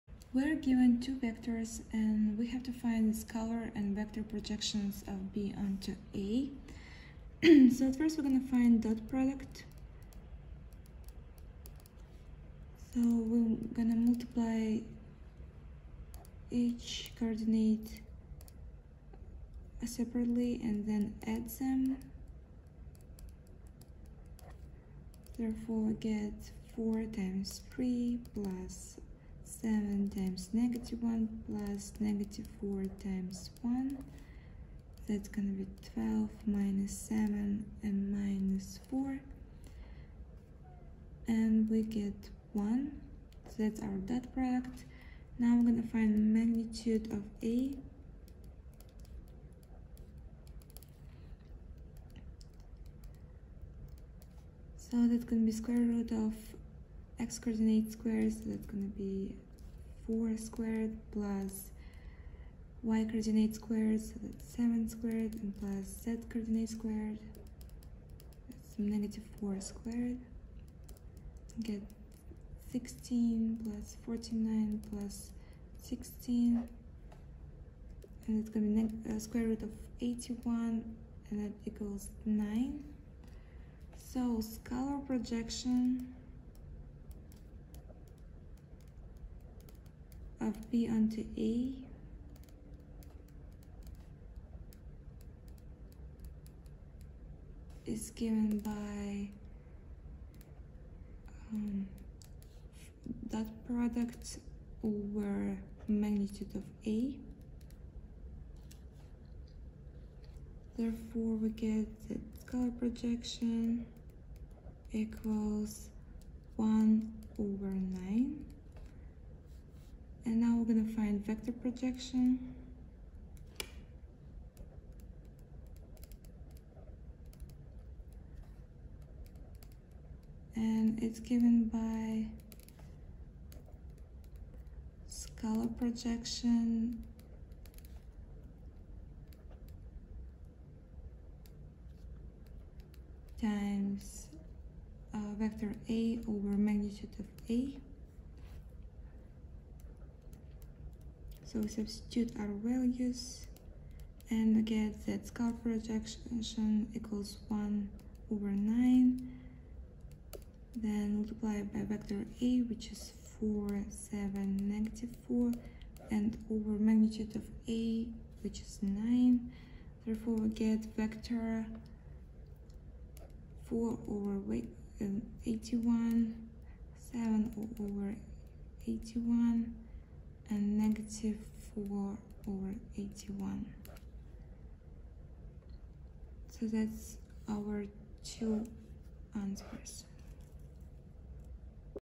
Find the scalar and vector projections of b onto a. a=(4,7,-4), b=(3, -1, 1) | Plainmath
Solution to Calculus and Analysis question: Find the scalar and vector projections of b onto a. a=(4,7,-4), b=(3, -1, 1)